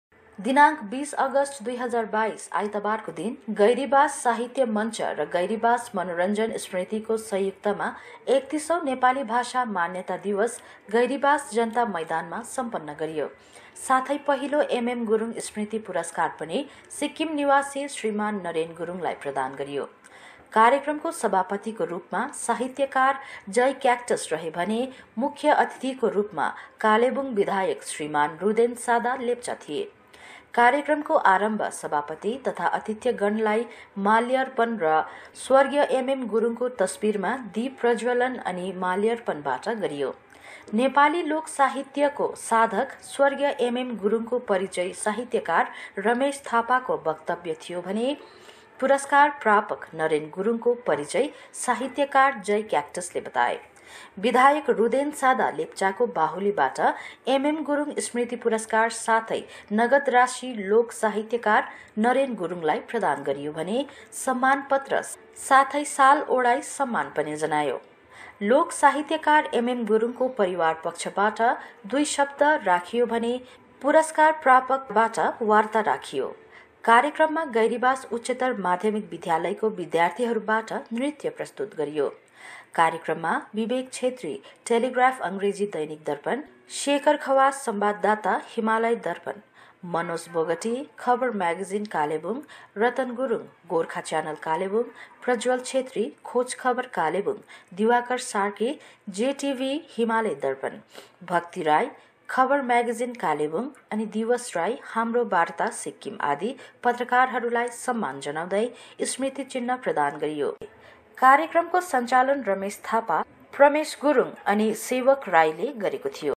31 औँ नेपाली भाषा दिवस पालन साथै पहिलो एम. एम. गुरुङ स्मृति पुरस्कार अलङ्करण समारोह
2022दिनांक 20 अगस्ट 2022 आइतबारको दिन गैरीबास साहित्य मञ्च र गैरीबास मनोरञ्जन समितिको संयुक्तमा 31 औँ नेपाली भाषा मान्यता दिवस गैरीबास जन्ता मैदानमा सम्पन्न गरियो साथै पहिलो एम. एम. गुरुङ स्मृति पुरस्कार पनि सिक्किम निवासी श्रीमान नरेन गुरुङलाई प्रदान गरियो | कार्यक्रमको सभापतिको रुपमा साहित्यकार जय क्याक्टस रहे भने मुख्य अतिथिको रूपमा कालेबुंग बिधायक श्रीमान रुदेन सादा लेप्चा यसरी नै अन्य अतिथिहरू 44/45 समष्टिका सभासद र साहित्यकारहरू उपस्थित थिए | कार्यक्रमको आरम्भ सभापति तथा अतिथिगणलाई माल्यार्पण र' स्व. एम. एम. गुरुङको तस्बीरमा दीप प्रज्वलन अनि माल्यार्पणबाट गरियो | उक्त कार्यक्रममा स्वाग्त भाषण साहित्य मञ्चका सचिव सेवक राईबाट गरियो भने भाषा मान्यता दिवसको औचित्य बारे दुई शब्द गैरीबास उच्चतर माध्यमिक विद्यालयको प्रधान अध्यापक श्रीमान पदम सुवेदीबाट राखियो | नेपाली लोक साहित्यको साधक स्व. एम. एम. गुरुङको परिचय साहित्यकार रमेस थापाको वक्तव्य थियो भने पुरस्कार प्रापक नरेन गुरुङको परिचय साहित्यकार जय क्याक्टसले बताए | यसरी नै बिधायक रुदेन सादा लेप्चाको बाहुलीबाट एम. एम. गुरुङ स्मृति पुरस्कार साथै नगद रासी लोक साहित्यकार नरेन गुरुङलाई प्रदान गरियो भने सम्मान पत्र साथै स्वाल अोढाई सम्मान पनि जनायो | लोक साहित्यकार एम. एम. गुरुङको परिवार पक्षबाट दुई शब्द राखियो भने पुरस्कर प्रापक कार्ताबाट दुई कुरा राखियो | उक्त कार्यक्रममा क्रमैले अनुष्कर पुष्कर गुरुङ, निरु बिन्दु, पदम सुवेदी अनि महेन्द्र मोक्तान आदि स्थानीय कविहरूले आफ्ना कविता बाचन गरियो भने गैरीबास उच्चतर माध्यमिक विद्यालयको विद्यार्थीहरूबाट सुन्दर सुन्दर नृत्य प्रस्तुत गरियो | उक्त कार्यक्रममा विवेक छेत्री ( टेलीग्राफ अंग्रेजी दैनिक दर्पण ), शेखर खवास ( सम्वाददाता हिमाल्य दर्पण ) ,मनोज बोगटी ( खबर म्य्गेजिन कालेबुङ ), रतन गुरुंङ ( गोर्खा च्यानल कालेबुङ ), प्रज्वल छेत्री ( खोज खबर कालेबुङ ), दिवाकर सार्की ( जे. टी. भी. / हिमाल्य दर्पण ) ,भक्ति राई ( खबर म्यागेजिन कालेबुङ ) अनि दिवस राई ( हाम्रो वार्ता सिक्किम) आदि पत्रकारहरूलाई सम्मान जनाउँदै स्मृति चिन्ह प्रदान गरियो  | उक्त कार्यक्रमलाई रोचक बनाउन क्रमैले पाल्मू तामाङ,गौरी राई, रजनी राई, सोनू सूनार, सलिम गुरुङ,अर्पणा शेर्पाले र' अमिसा वार्सकर्मले गीत प्रस्तुत गरे | उक्त कार्यक्रमको सञ्चालन रमेस थापा, प्रमेस गुरुङ अनि सेवक राईले गरियो भने मुख्य अतिथिको सम्भाषण पनि छोटो राखियो  | कार्यक्रमको अन्त धन्यवाद ज्ञापन प्रसाद रनपहेंलीबाट गरियो भने सभापति जय क्याक्टसबाट सभा समापनको घोषणा गरियो |खबर सौजन्य : अनुष्कर पुष्कर गुरुङ ( गैरीबास )